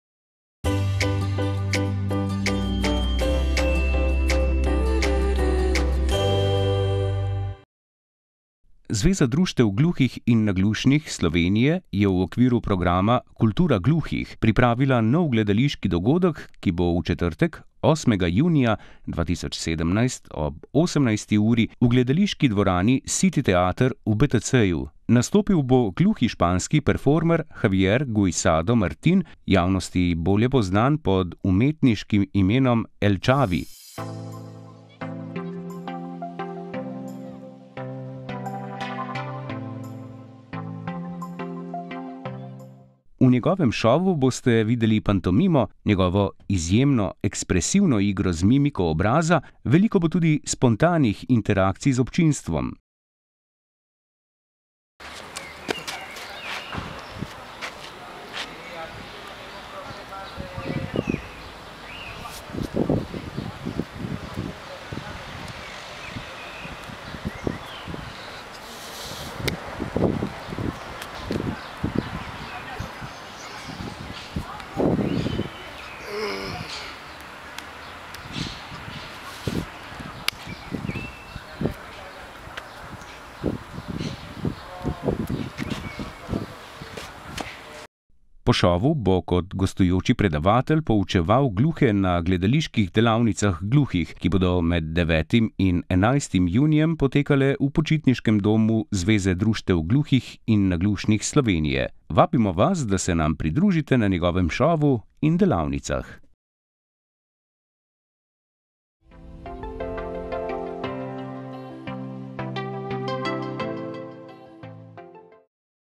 Nastop El Chavija v Sloveniji in gledališka delavnica
Španski gluhi performer El Chavi bo 8. junija 2017 ob 18h nastopil v SiTi Teatru. V El Chavi Šovu boste videli pantomimo, njegovo izjemno ekspresivno igro z mimiko obraza, veliko bo tudi spontanih interakcij z občinstvom. Njegovi nastopi so dosegli velik uspeh tako v Španiji in drugod po Evropi kot tudi v Združenih državah Amerike. Vstop je prost!